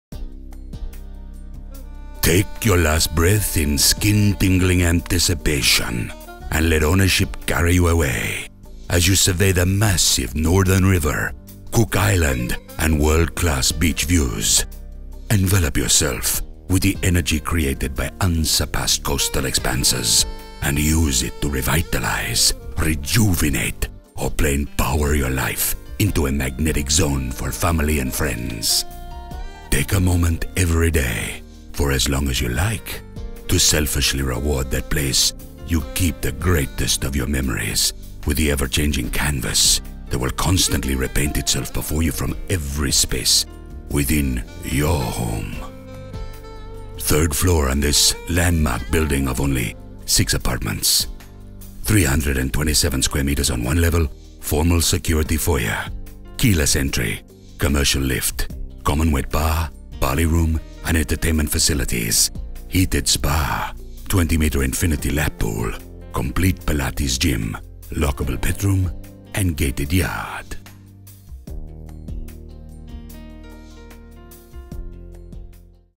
3/13 Eden Avenue, Tweed Heads QLD By Scott Wagner
Property Video shoot of 3/13 Eden Avenue, Tweed Heads QLD by PlatinumHD for Ray White Mermaid Beach